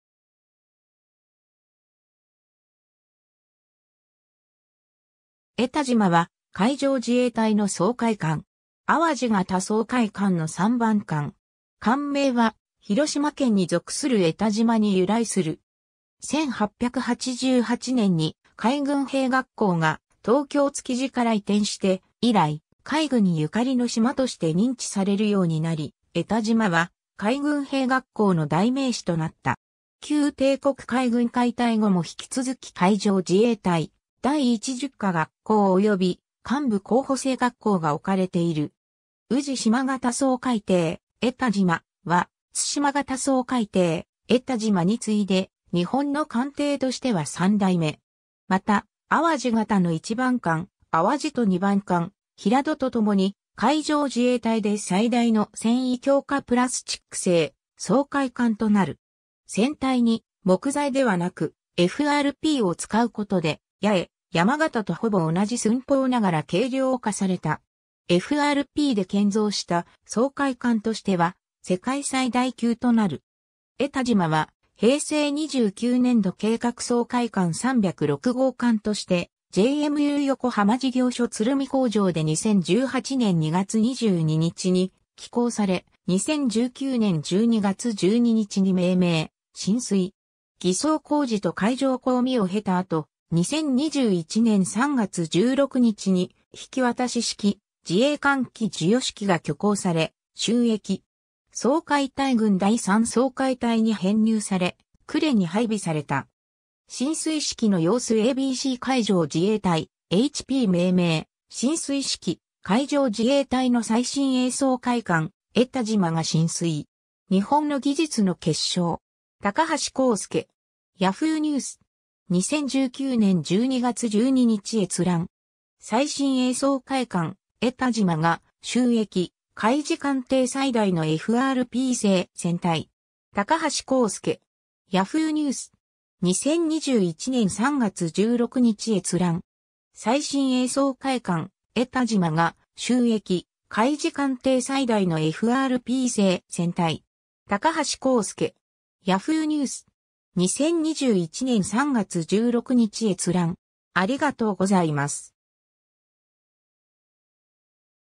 えたじま (掃海艦)
えたじま（ローマ字：JS Etajima, MSO-306）は、海上自衛隊の掃海艦。
あわじ型掃海艦の3番艦。
艦名は広島県に属する江田島に由来する。
1888年（明治21年）に海軍兵学校が東京築地から移転して以来、海軍ゆかりの島として認知されるようになり、「江田島」は海軍兵学校の代名詞となった。
旧帝国海軍解体後も引き続き海上自衛隊第1術科学校及び幹部候補生学校が置かれている。
うじしま型掃海艇「えたじま」、はつしま型掃海艇「えたじま」に次いで日本の艦艇としては3代目。
また、あわじ型の1番艦「あわじ」と2番艦「ひらど」とともに海上自衛隊で最大の繊維強化プラスチック（FRP）製掃海艦となる。
船体に木材ではなく、FRPを使うことで、やえやま型とほぼ同じ寸法ながら軽量化された。
FRPで建造した掃海艦としては世界最大級となる。
 「えたじま」は、平成29年度（2017年度）計画掃海艦306号艦として、JMU横浜事業所鶴見工場で2018年2月22日に起工され、2019年12月12日に命名・進水。
艤装工事と海上公試を経た後、2021年3月16日に引渡式・自衛艦旗授与式が挙行され、就役。
掃海隊群第3掃海隊に編入され、呉に配備された。
 進水式の様子 ^ a b c 海上自衛隊HP 命名・進水式 ^ “海上自衛隊の最新鋭掃海艦「えたじま」が進水。日本の技術の結晶”. 高橋浩祐. Yahoo!ニュース. 2019年12月12日閲覧。
 ^ “最新鋭掃海艦「えたじま」が就役――海自艦艇最大のFRP製船体”. 高橋浩祐. Yahoo!ニュース. 2021年3月16日閲覧。